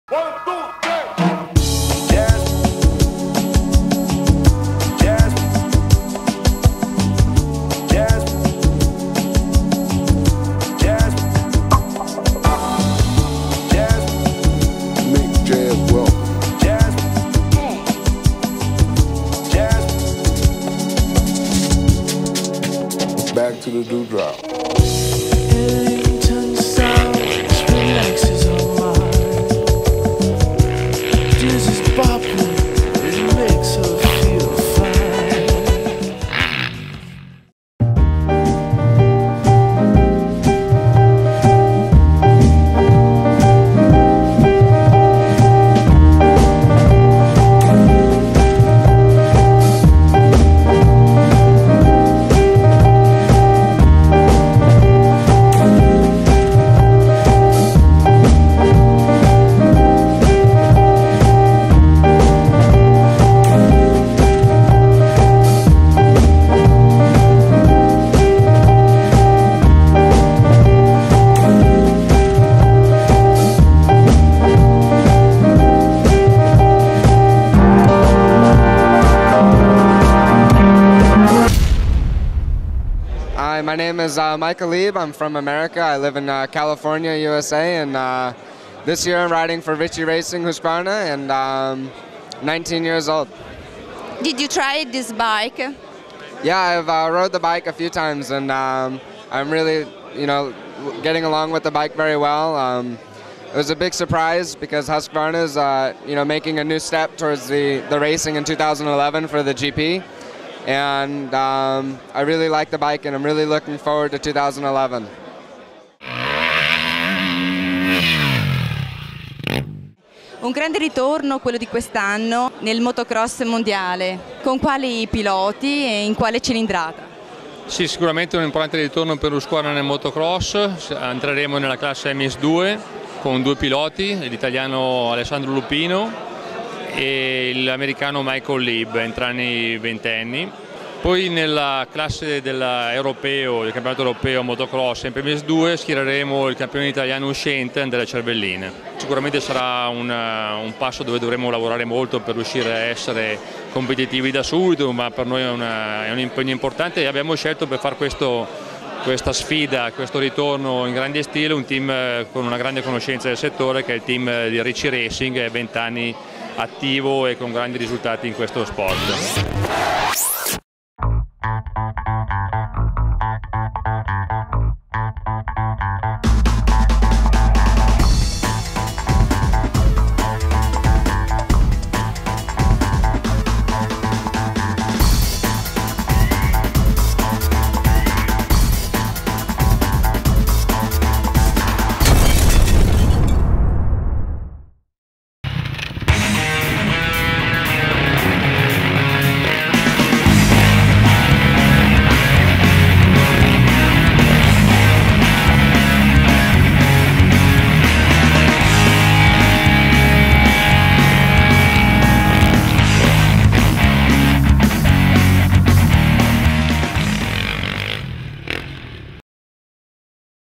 Husqvarna Racing MX2 World Championship Challenge 2011
Husqvarna has a great tradition within the world motocross scene, achieving nothing less than 21 world titles in this discipline, that is rightly considered the F1 of off road motorcycling.
In 2011 Husqvarna returns to the world MX stage with an official entry in the FIM MX2 World Championship. Husqvarna's last official appearance at world motocross racing with a small capacity bike was back in 2003 with the 125 two stroke. For this latest adventure, Husqvarna has chosen a Team with extensive experience in world championship motocross: Team Ricci Racing.
Ricci's riders for the Husqvarna assault on the MX2 Championship are Alessandro Lupino and Michael Leib. In a team headed by team manager Ilario Ricci, they will campaign all 15 rounds of the World Championship as well as the 4 races of Italian Motocross International Championship.
Andrea Cervellin, the young Italian motocross champion in 2010, will complete the team. This fresh rider is back from a knee surgery and will participate again in the European Championship MX2, with aim of improving on his second place achieved last season riding Husqvarna TC250.
Team Ricci Racing has been developing the TC250 for several months now, working in conjunction with Paolo Brovedani, chief engineer of Husqvarna R&D Department, Pierluigi Zampieri, Husqvarna racing department manager and Ralf Kleid, Husqvarna engine R&D manager.